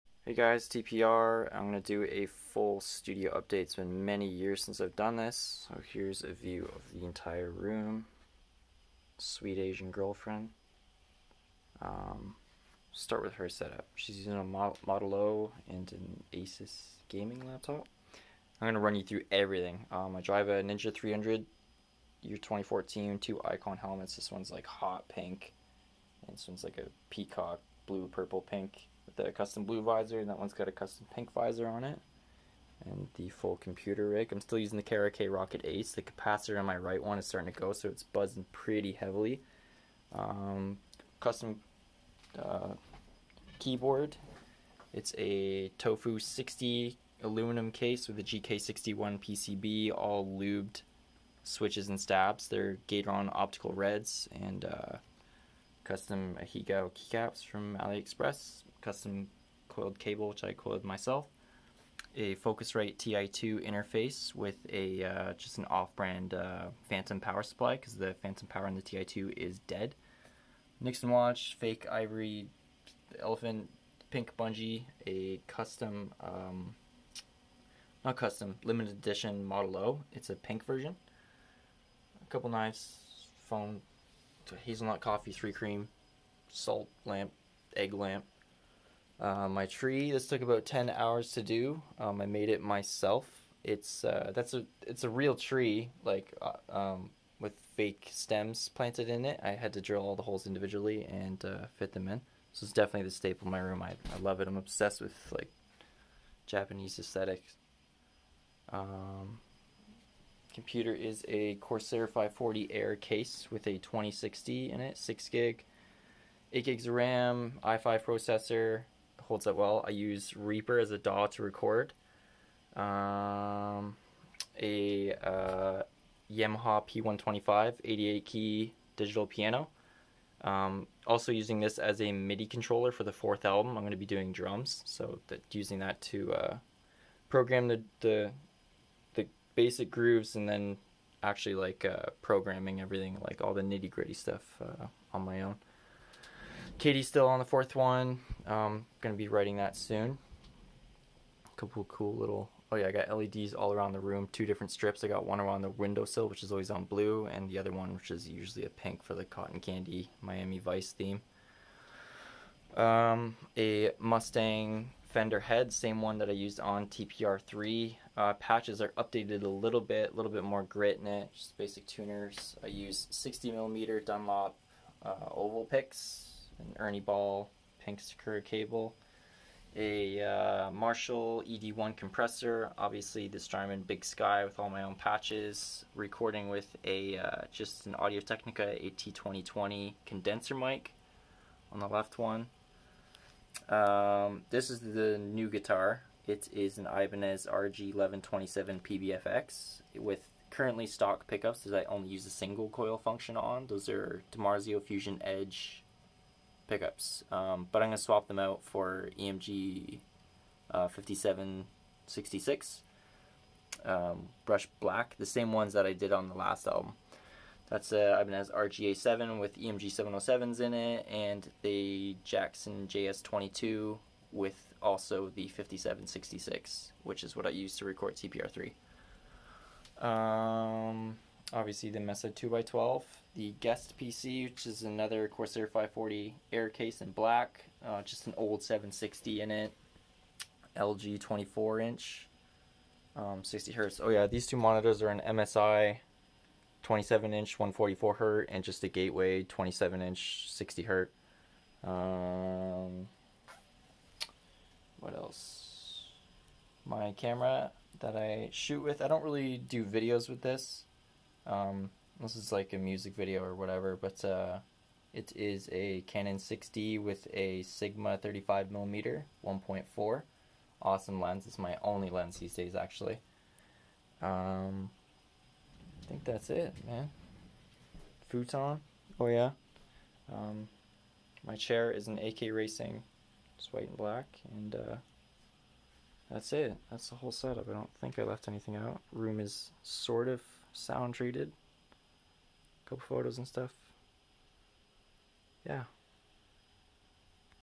TPR Studio Tour 2020
Been a long time, I know. My youtube days are pretty over, but here's an update on my setup and to let you know I'll be starting TPR4 soon.